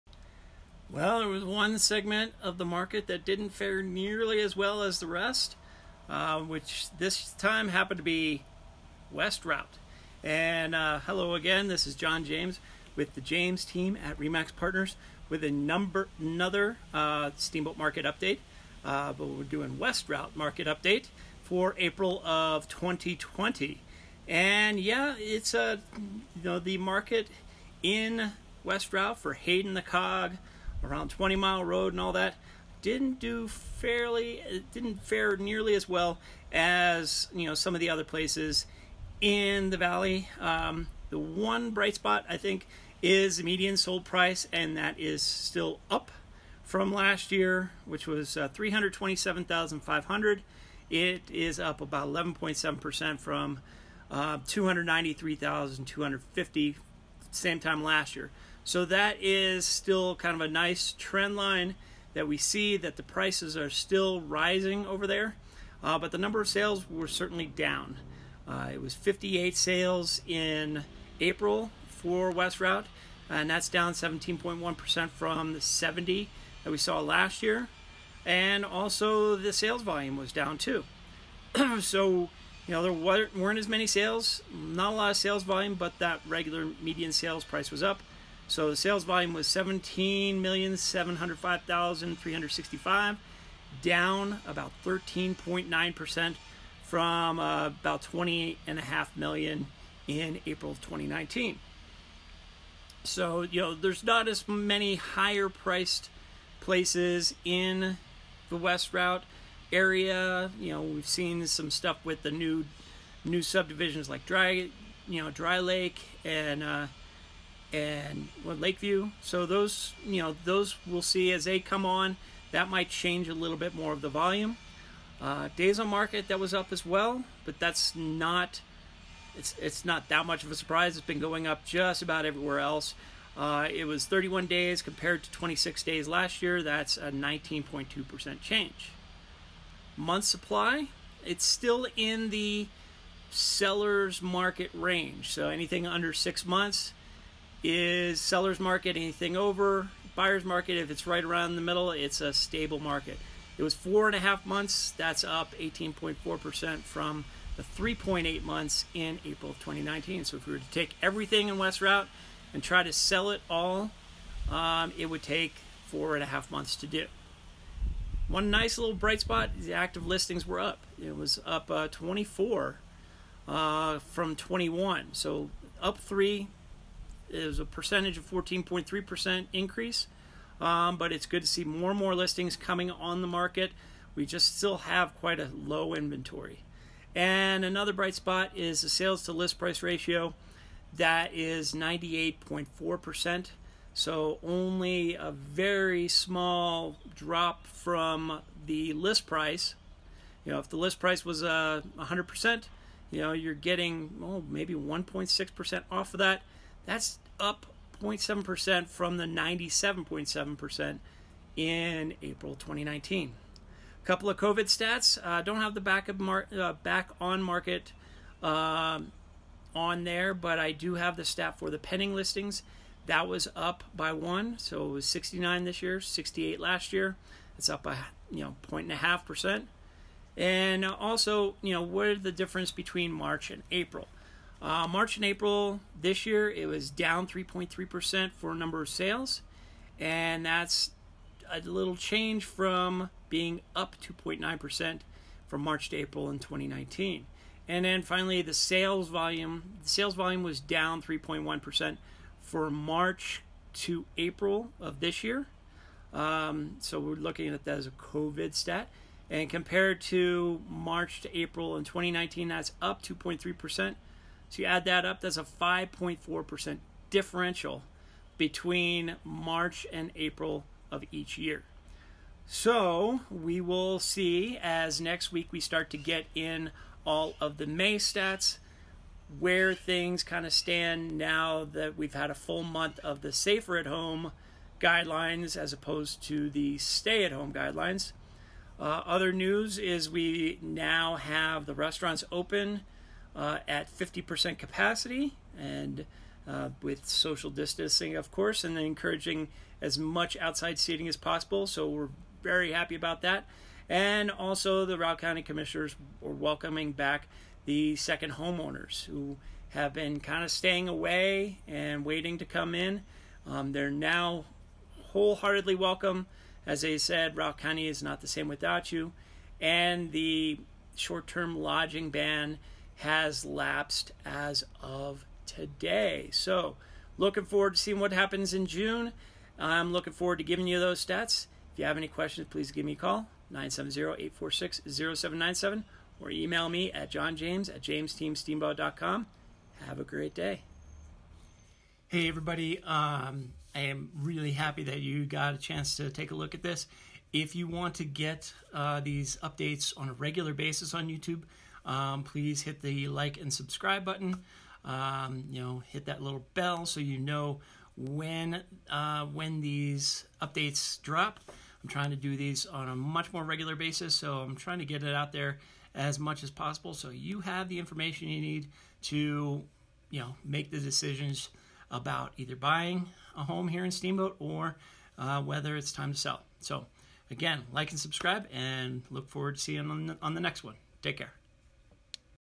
How's the Market? April 2020 West Routt Edition
Not exciting news in West Routt, but not terribly awful as well. Median Sales Price was up from last year, but most of the other metrics were down. Last of the Market Updates for the month of April. Looking forward to seeing how the market fared in May. Starting those videos next week!